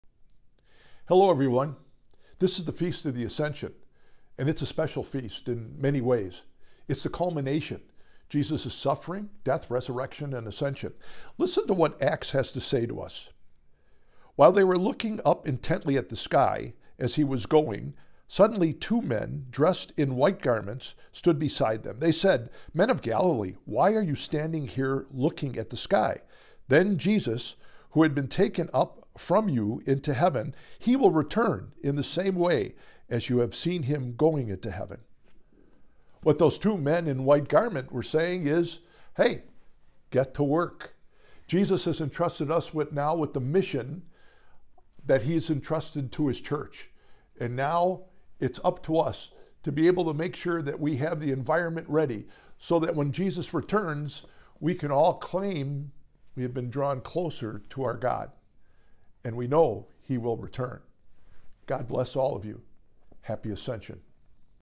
The Good News 05.08.2024
DON’T JUST STAND THERE — Archbishop Listecki is eager to share with you a brief, encouraging message about the upcoming celebration of the Ascension of the Lord! Check out The Good News today.